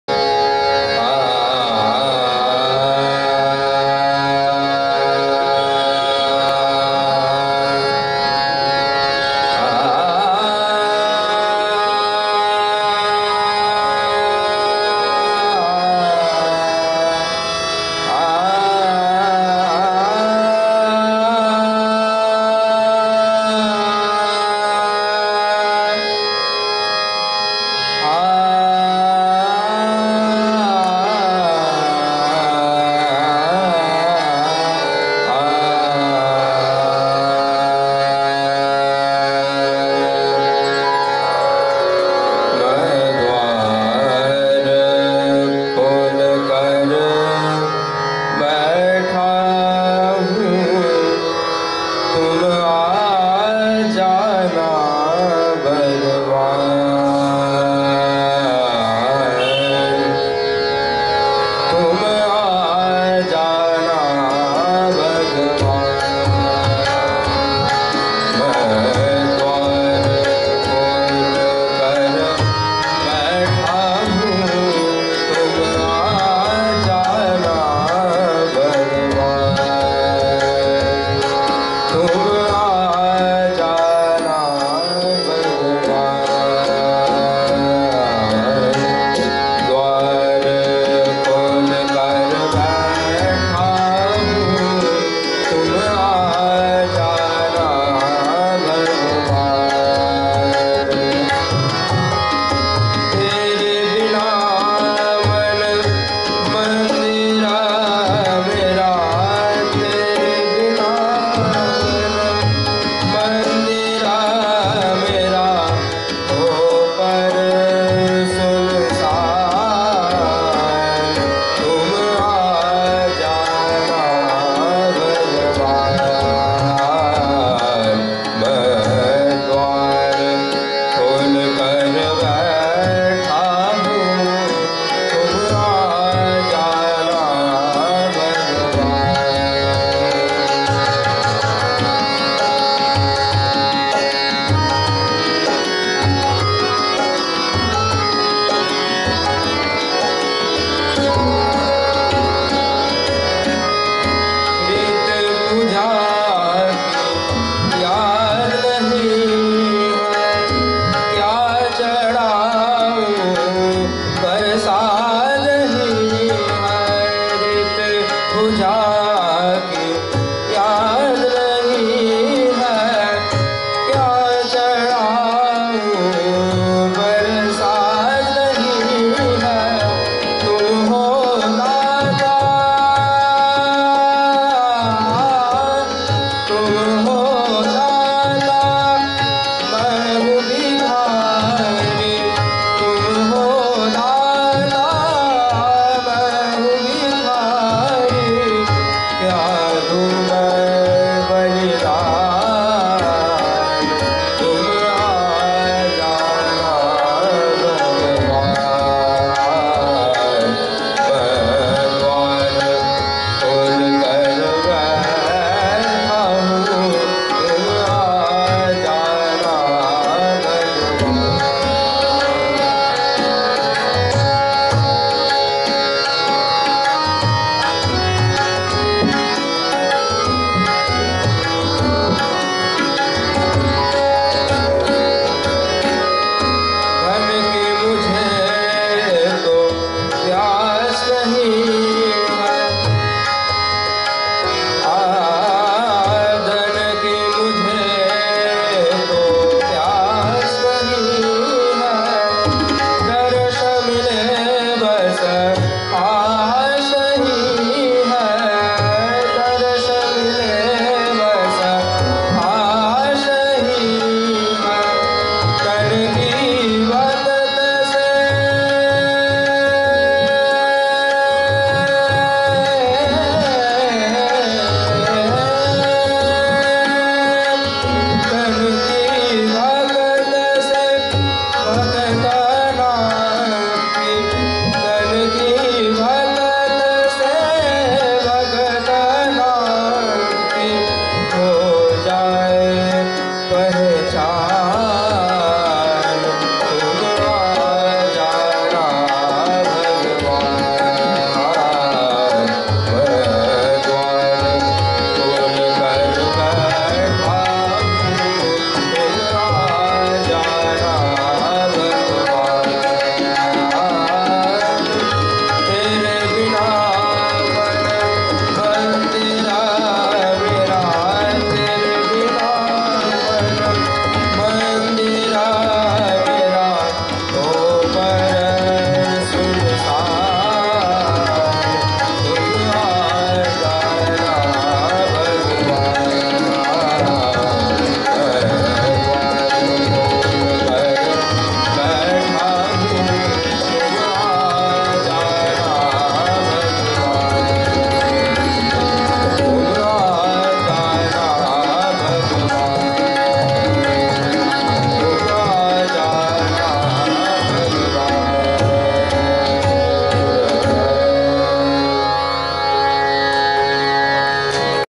Dhrubadyuti Haldar Vocal Bhajan Tabla Subrata Das Harmonium Basudeb banerjee
Dhrubadyuti, Subrata and Basudeb live on 19,3,2022 at Sibtala Durgagati Memorial Hall at GOPON AAKASH's MARGASANGEETER AASAR